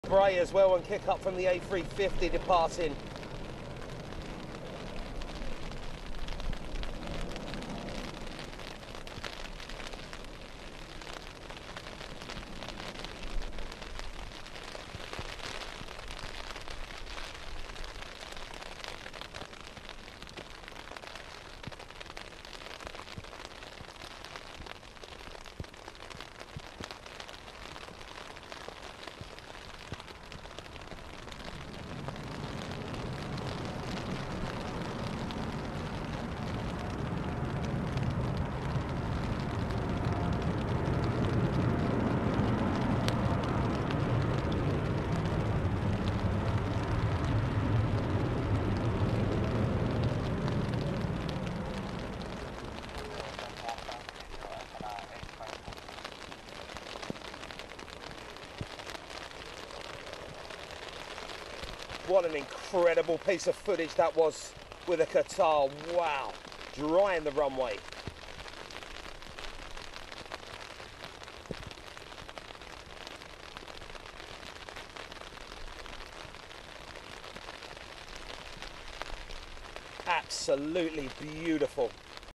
Spray
Another fantastic head on look at the 787 arriving on runway 26L this was one of the wettest days of the summer. Check out the incredible spray